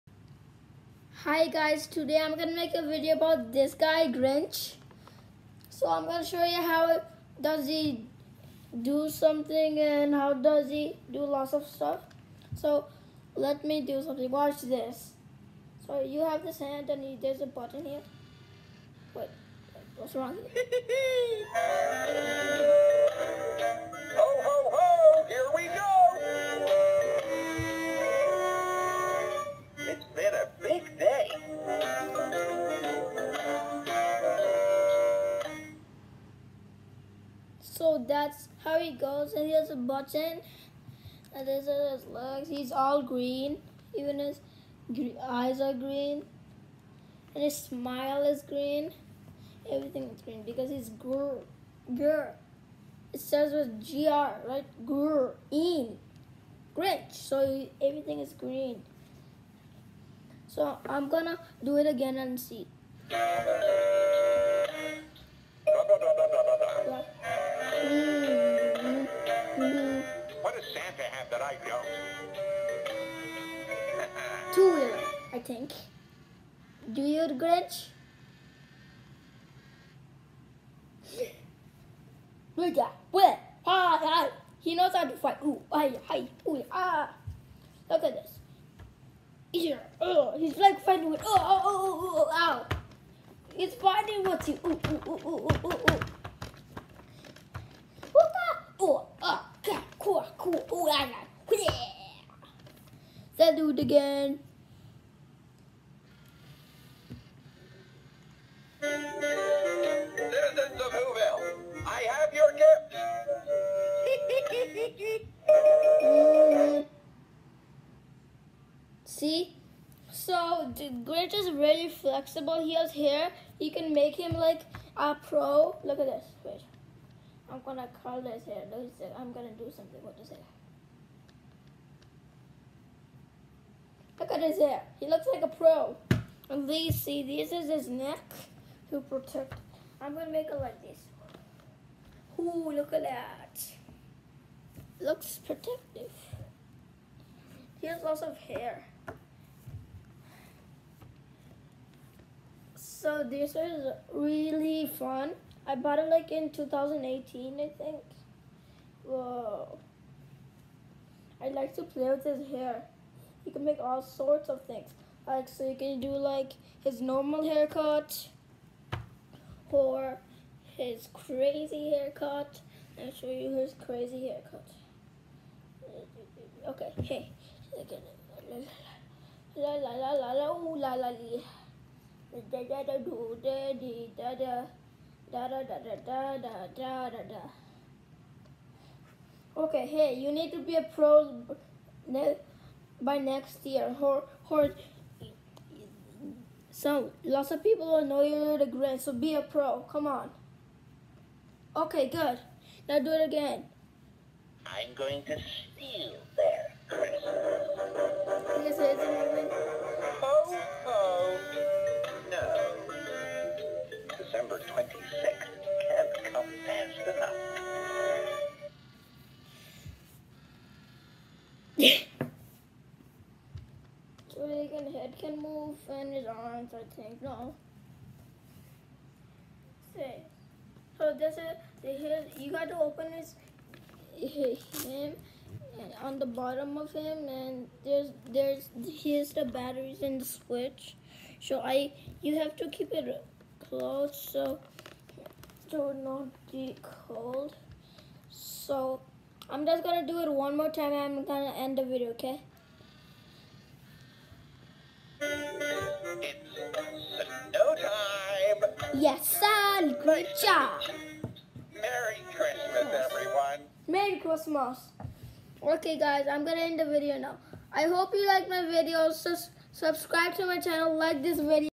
Grinch review
I saw the grinch movie and I I loved  it. Hope u will enjoy watching my grinch toy review .